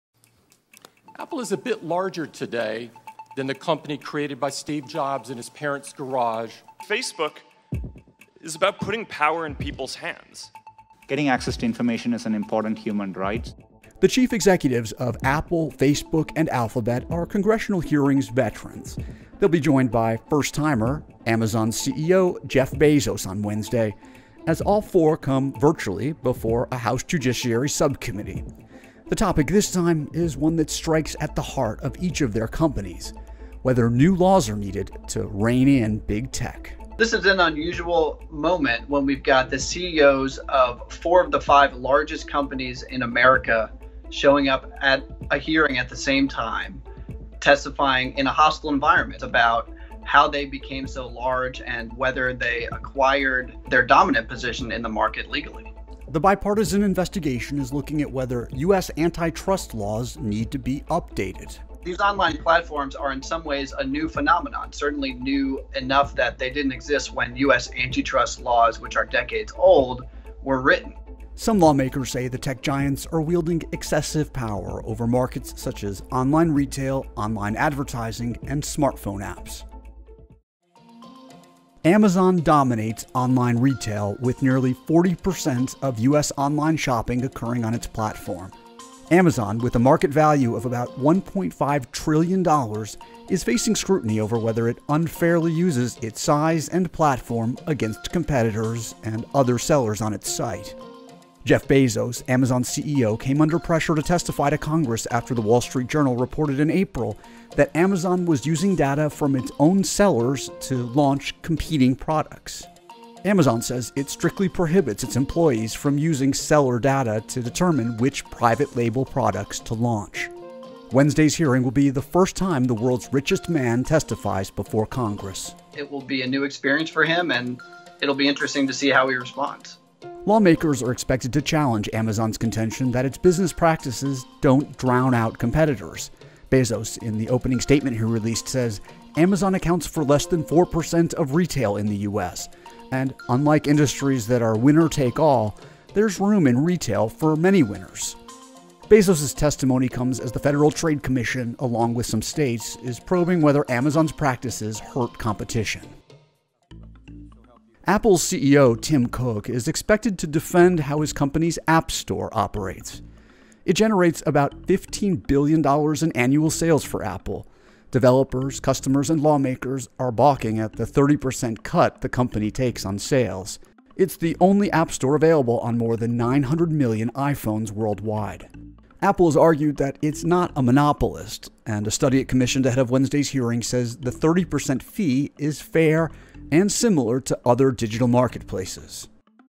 Oral de langue - Anglais - Exemple 13
Bezos, Cook, Zuckerberg and Pichai Face Lawmakers' Antitrust Concerns